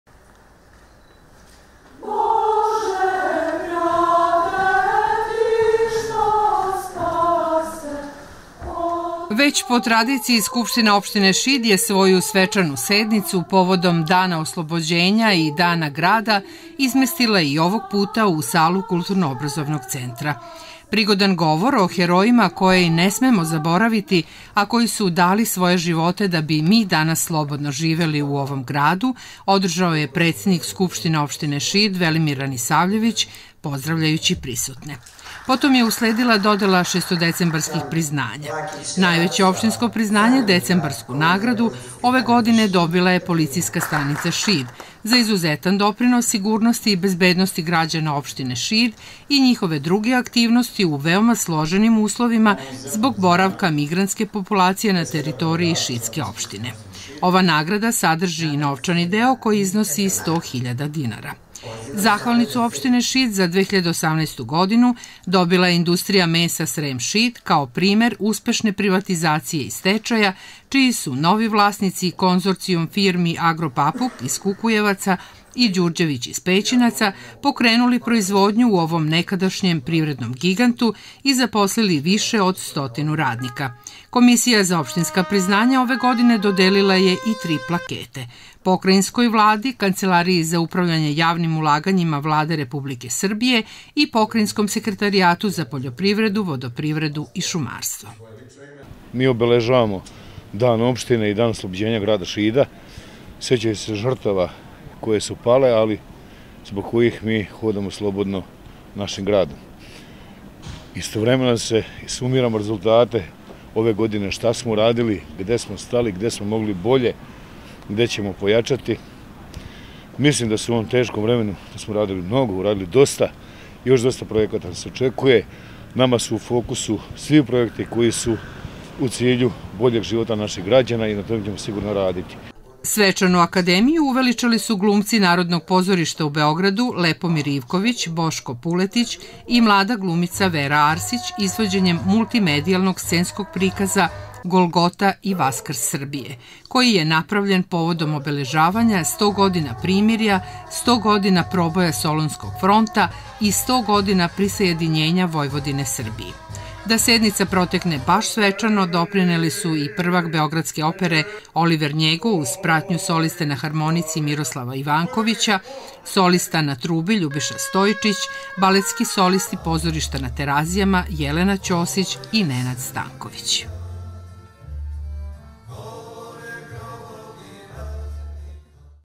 Svečana sednica SO Šid
Sinoć je u sali Kulturno obrazovnog centra održana svečana sednica Skupštine opštine Šid sa akademijom povodom Dana oslobođenja i Dana opštine koju je otvorio Velimir Ranisavljević, predsednik Skupštine opštine Šid. Potom je predsednik opštine , Predrag Vuković, uručio nagrade ovodišnjim dobitnicima šestodecembarskog priznanja.